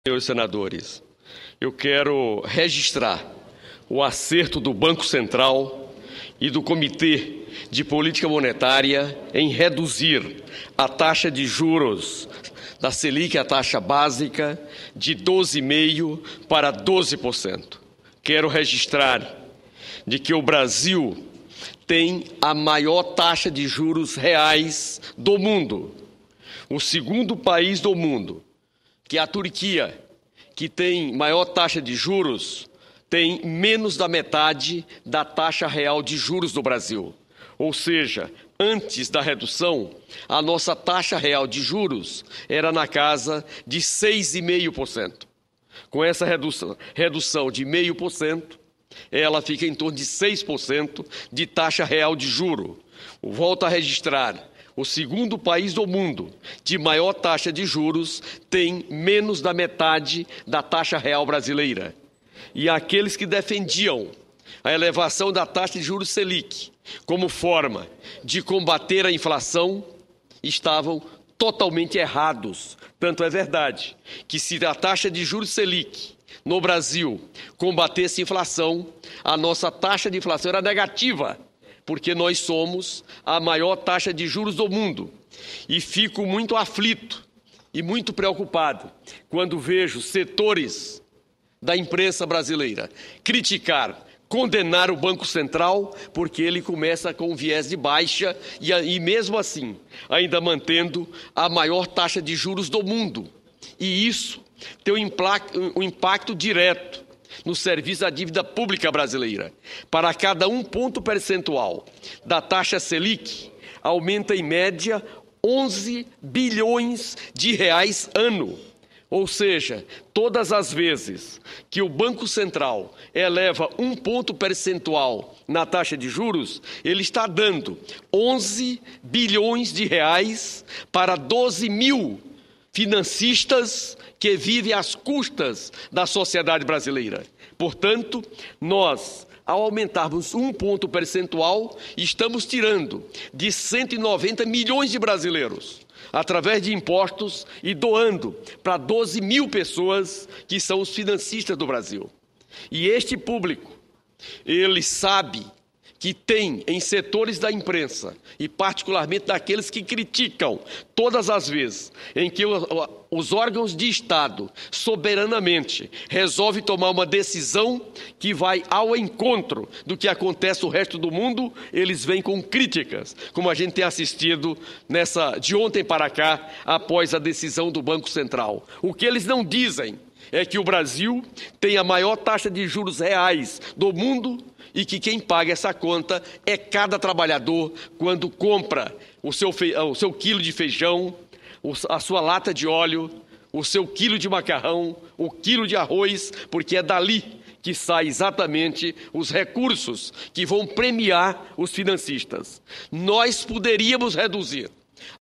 Sen. José Pimentel, PT/CE, felicita Comitê de Política Monetária pela redução da taxa de juros
01/09/2011 - Plenário - Sen. José Pimentel, PT/CE, felicita Comitê de Política Monetária pela redução da taxa de juros